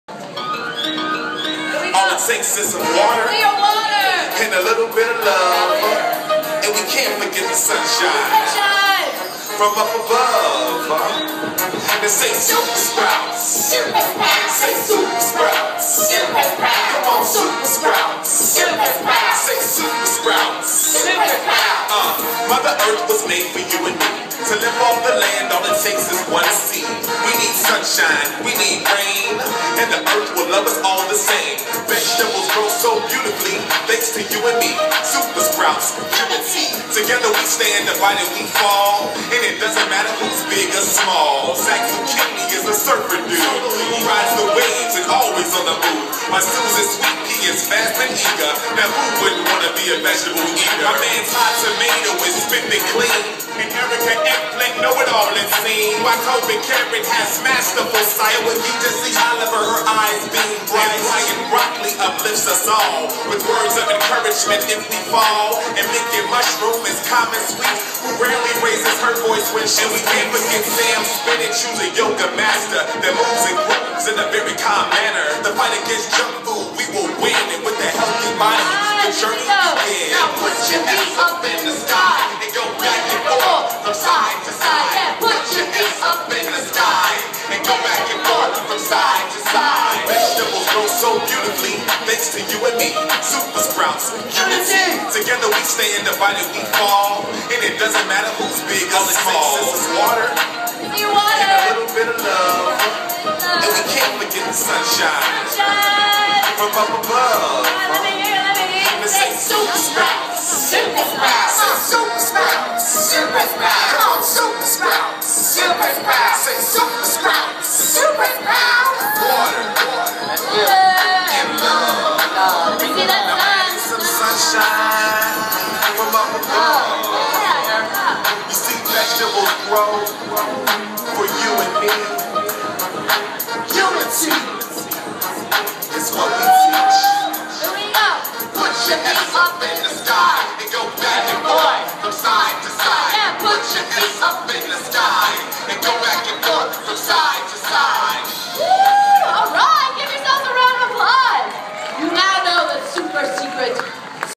Brian Broccoli
Brian Davie performing with super sprouts